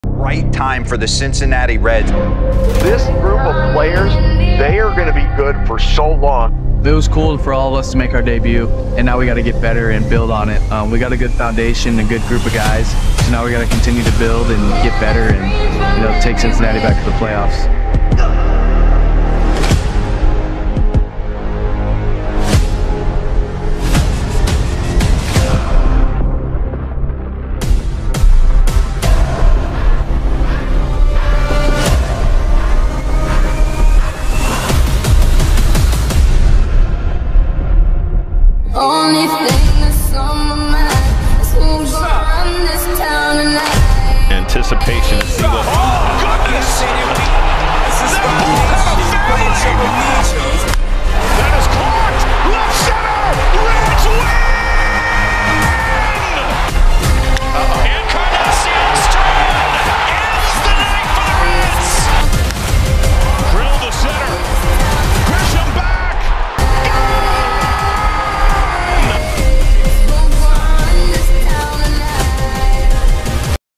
Cincinnati Reds hype video 2024
After a exciting season for these young rookies The Cincinnati reds are ready too make a big push for the playoffs and possibly the division GO REDS